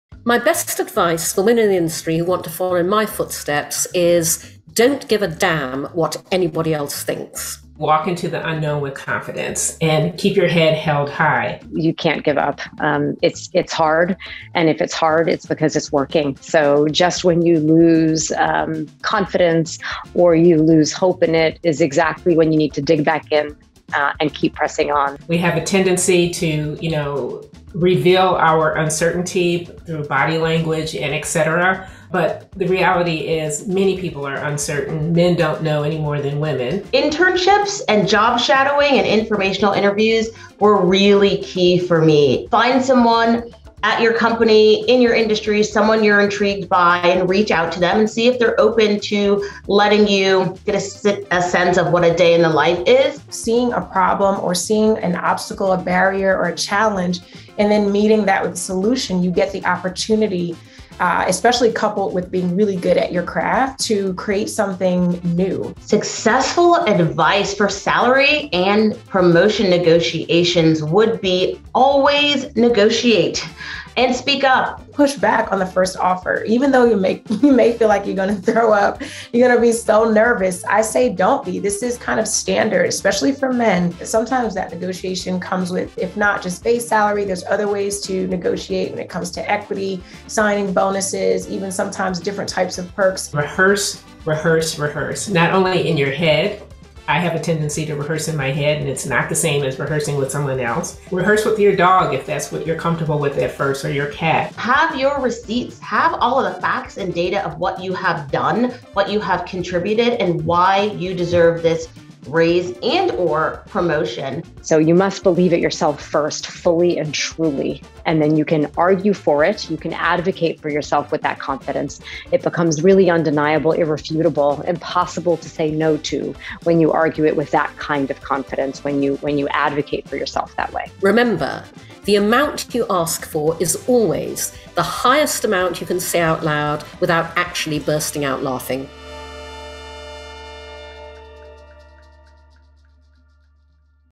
5 Women Marketing Leaders Share Their Best Career Tips
For Women’s History Month, Adweek asked five brand and agency executives to share their best career advice for women marketers.

We’re joined by Nancy Reyes, CEO of TBWA\Chiat\Day New York; God-is Rivera, global director of culture and community at Twitter; Cindy Gallop, advertising veteran and founder and CEO of Make Love, Not Porn; Kenya Brock, head of marketing and communications at We Are Rosie; and E.T. Franklin, chief strategy and cultural fluency officer at Spark Foundry.